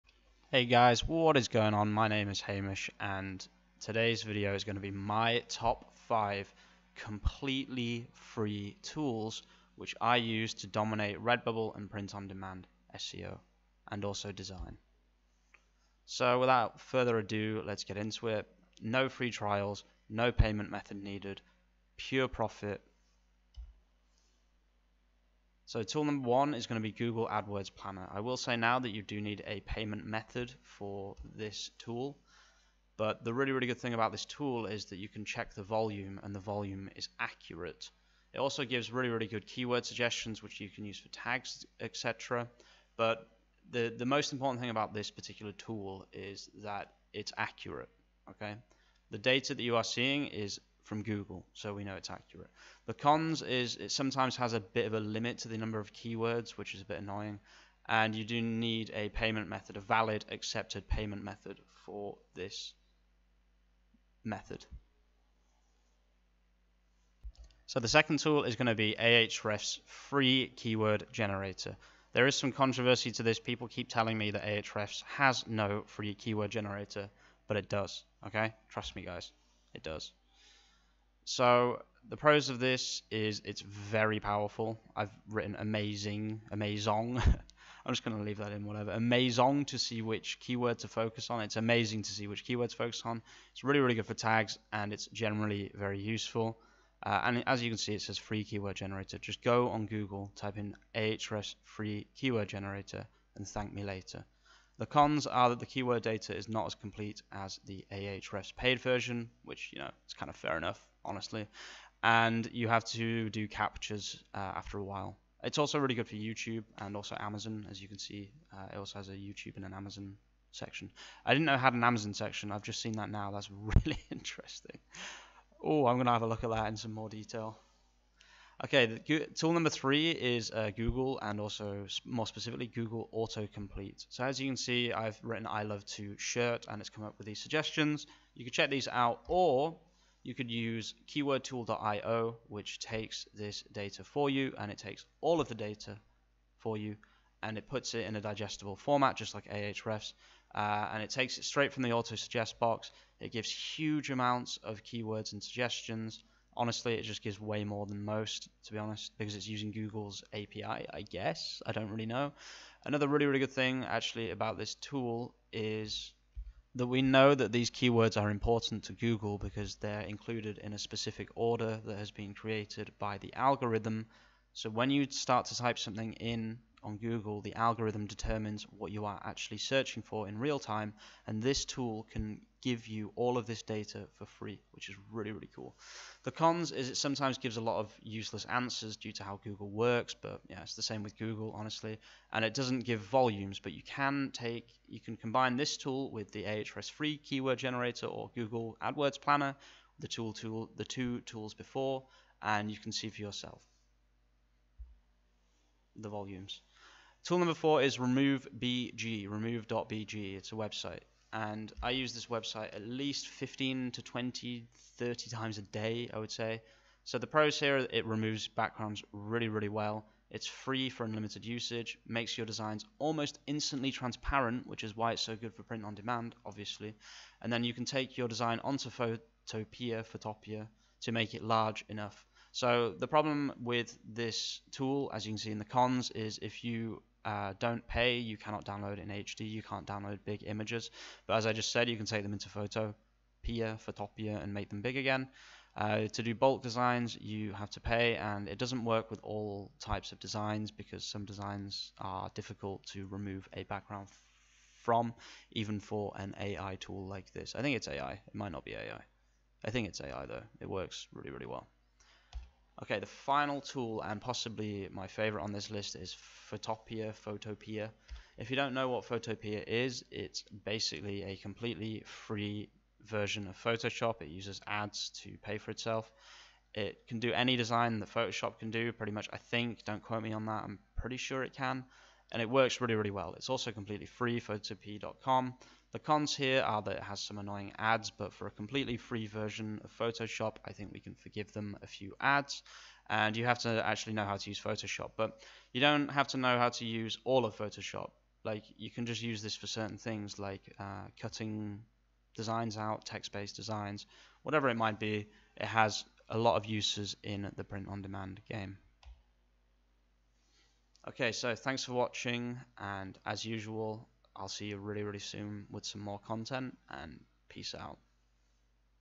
5 FREE TOOLS YOU CAN DOMINATE REDBUBBLE AND PRINT ON DEMAND WITH IN 2021/22
This video will give you a sneaky peak into my 5 secret tools to dominate redbubble and print on demand sales

The tools are all COMPLETELY FREE with no sign ups (except google adwords)

This means you can start using them to make money online today with no investment

Redbubble requires no investment, and is a great place to earn dollars from anywhere in the world

Best redbubble and print on demand keyword and design tools 2021/22

0:00 My top 5 completely free tools to dominate redbubble and other print on demand design and seo
0:28 Google Adwords planner - Amazing for getting accurate search data on websites
1:08 Ahrefs FREE keyword generator - check google and redbubble volumes to get accurate search data to base your designs on
2:08 Google Auto suggest/autocomplete tool and keywordtool.io - amazing redbubble print on demand tool
3:27 How to remove backgrounds from images in 5 seconds using remove.bg - make redbubble design transparent
4:24 Photopea - free version of photoshop - amazing for various design necessities